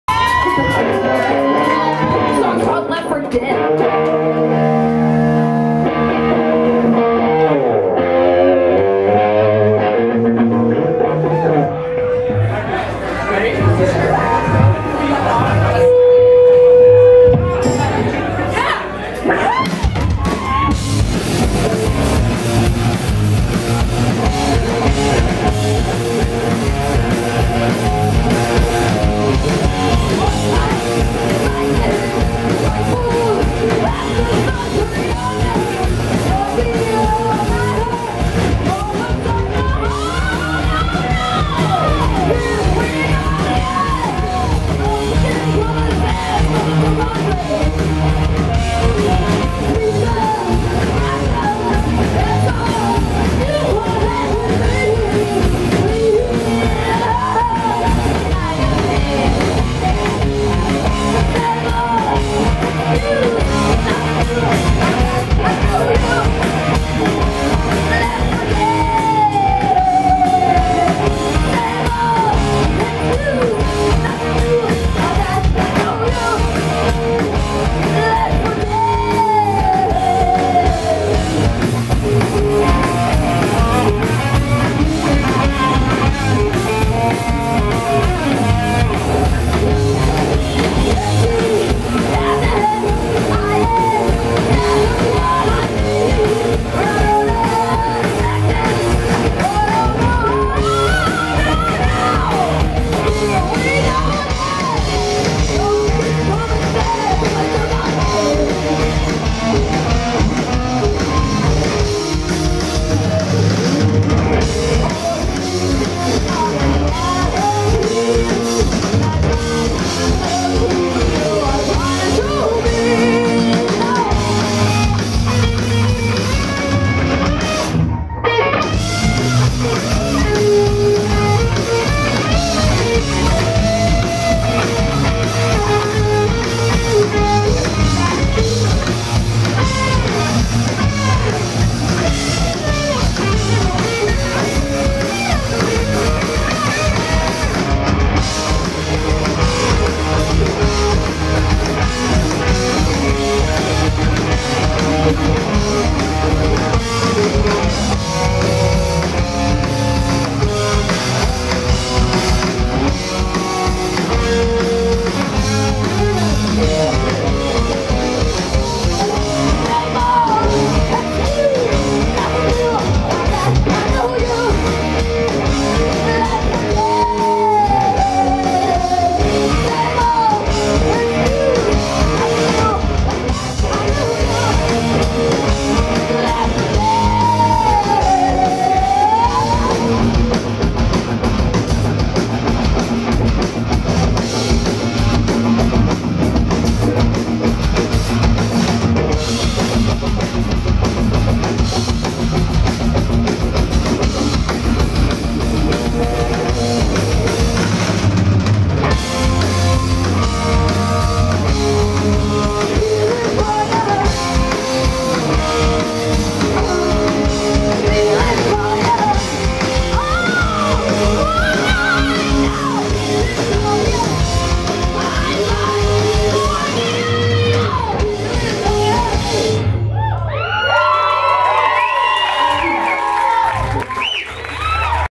Psychic Hit @ Bottom of the Hill 3
Hard rockin' proto-Metal sweethearts Psychic Hit whack the big stage at Bottom of the Hill, SF, 6th January 2018, for a crowded afternoon BBQ show ~ Fanx to the lit yobs at Subliminal SF and Iron Man Moving & Storage for throwing this bash ~ My last of three vids of the set ~ Pics at Boris Sev facebook ~